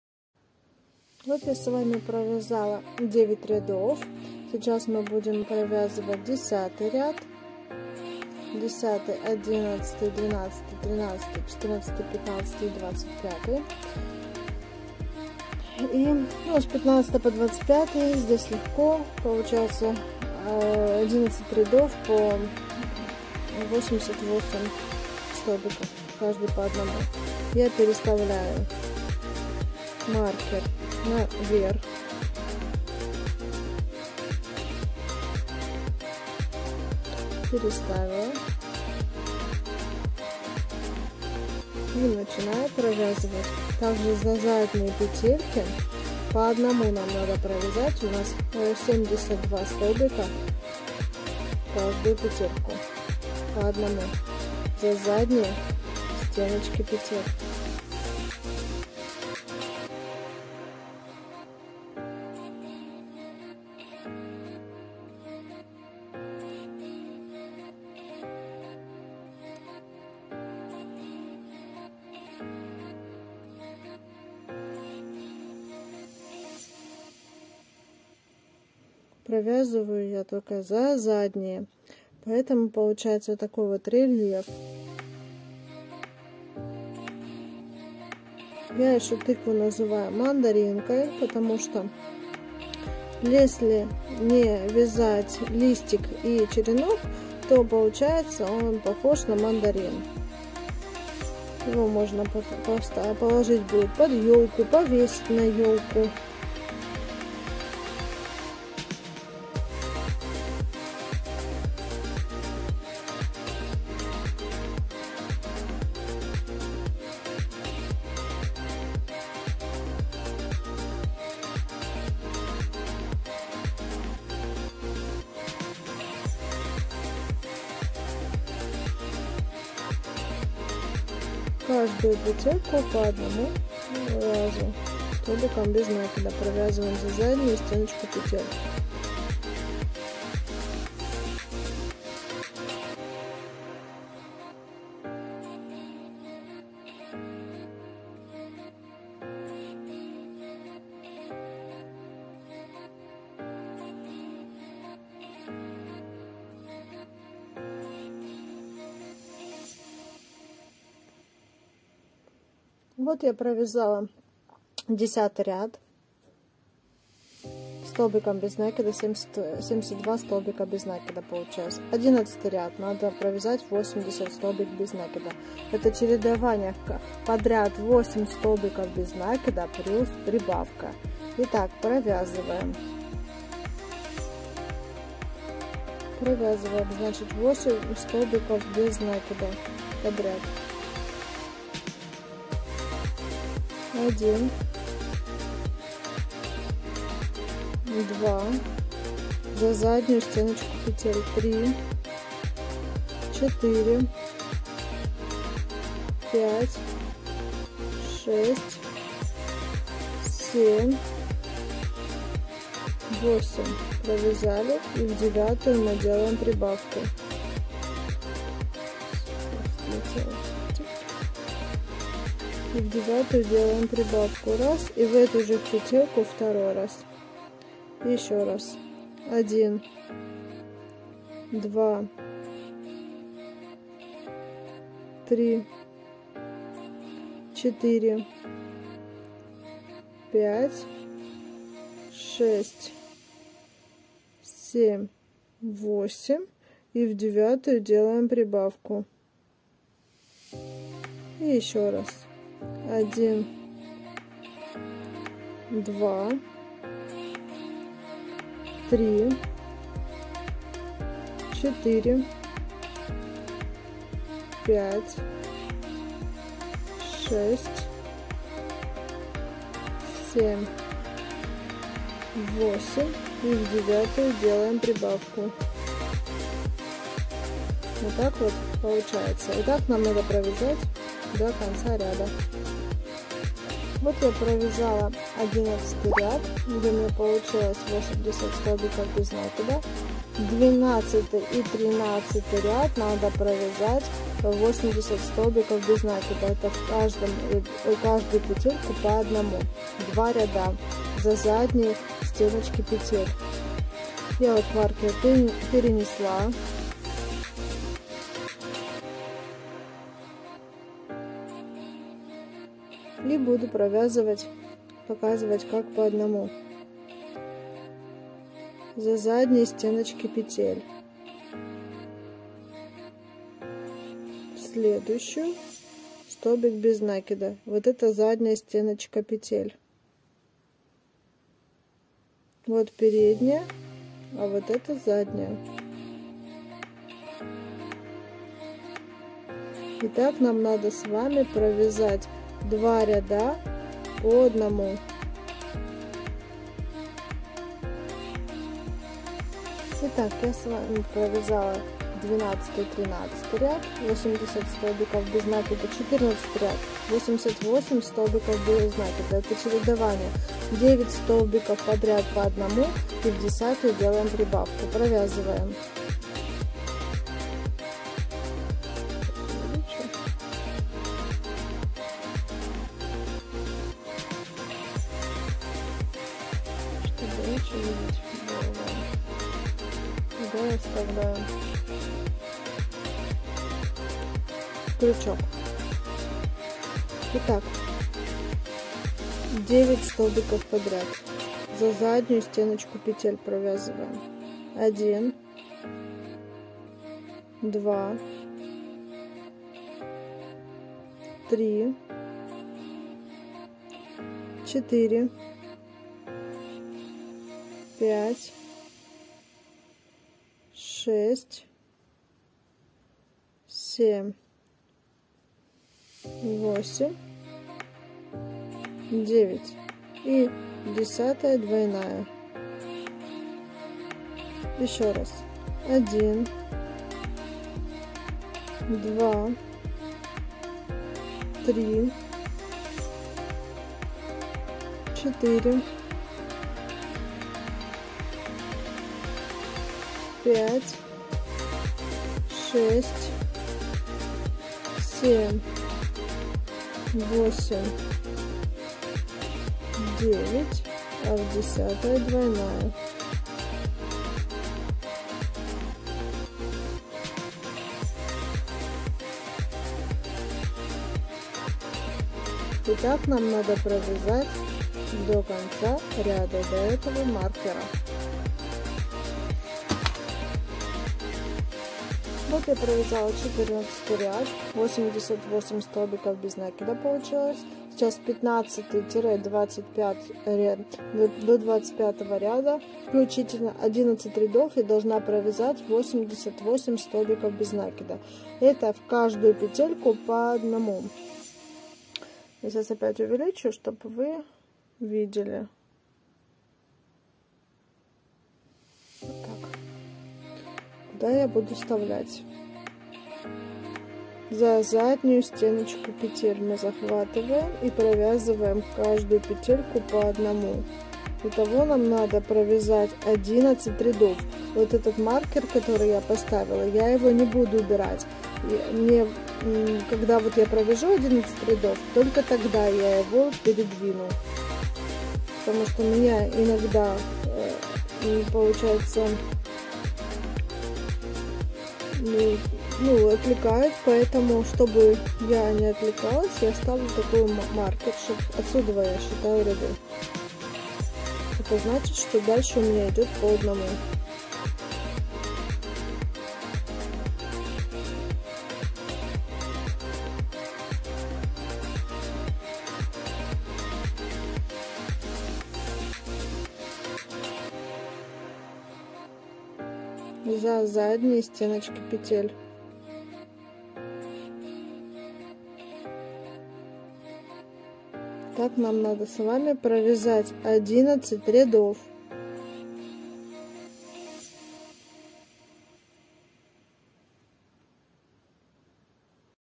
МАСТЕР-КЛАСС ЧАСТЬ 2. "ТЫКВА" ВЯЗАНИЕ КРЮЧКОМ
узор столбик без накида за заднюю стенку петли